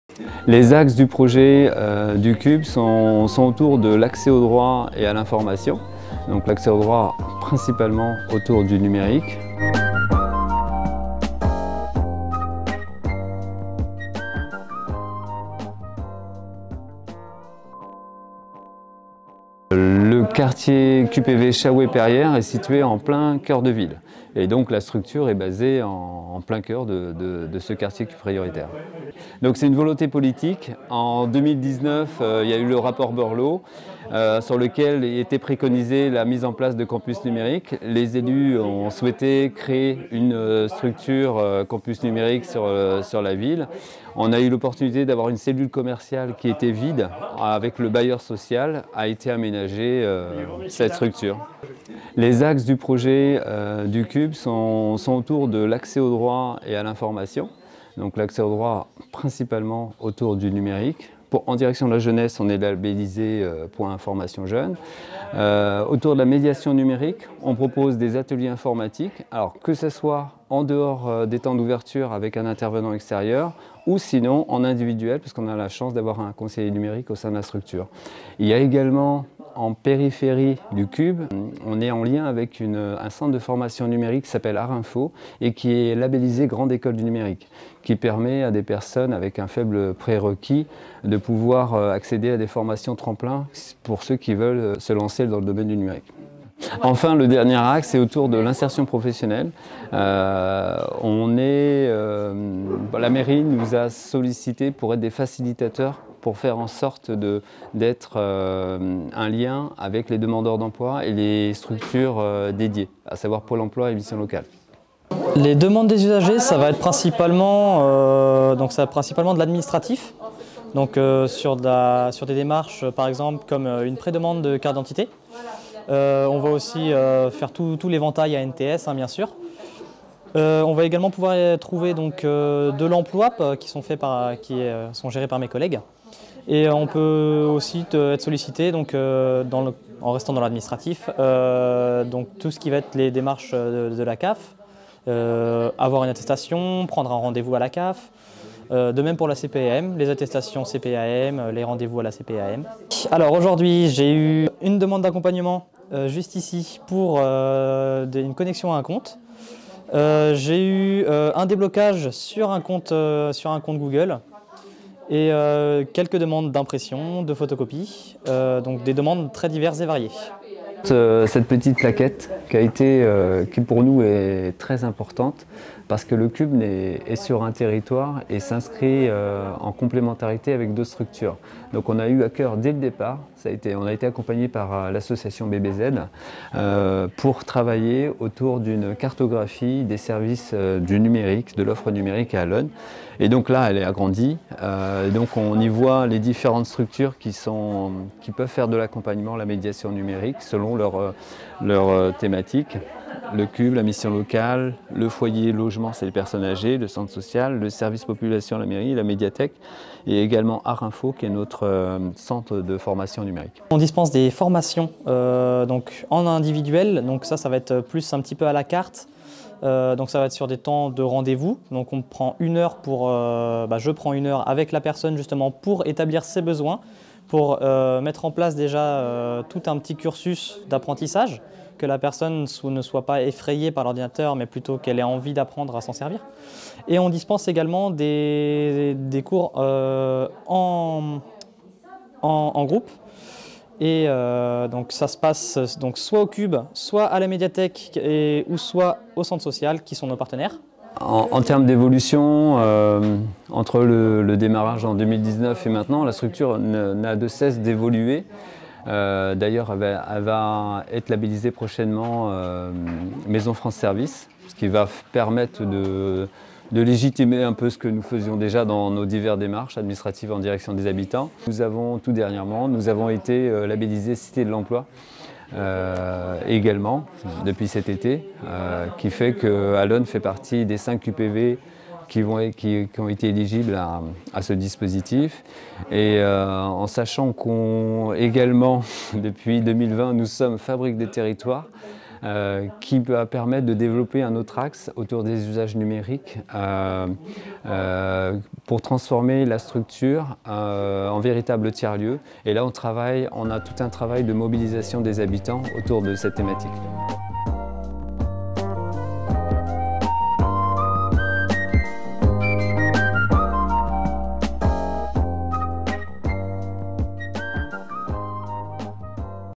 En immersion au Cube, campus numérique
Les axes du projet du Cube, à Allonnes, sont autour de l'accès aux droits et à l'information, principalement autour du numérique.